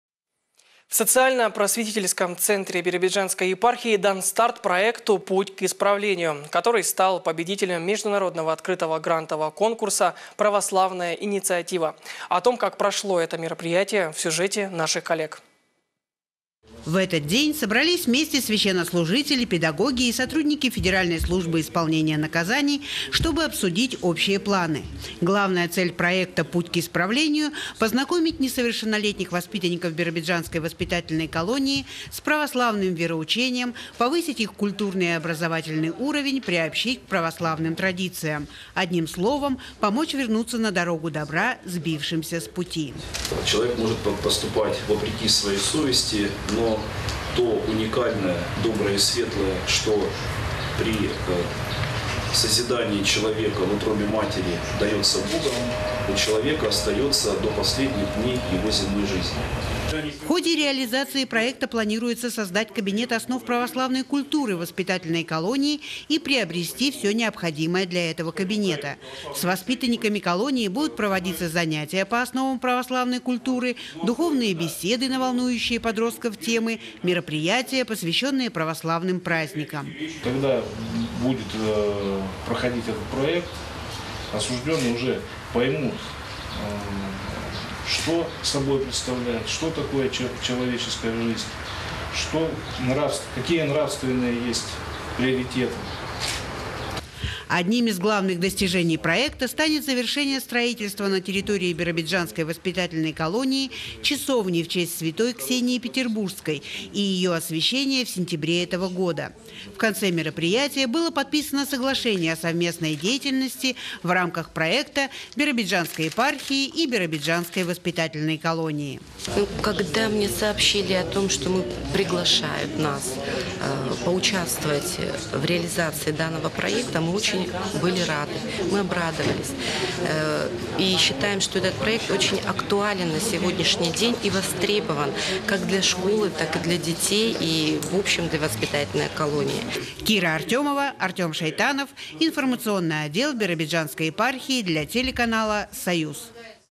В Биробиджанской епархии стартовал просветительский проект «Путь к исправлению»
В Социально-просветительском центре Биробиджанской епархии дан старт проекту «Путь к исправлению», который стал победителем Международного открытого грантового конкурса «Православная инициатива». Как прошло это мероприятие?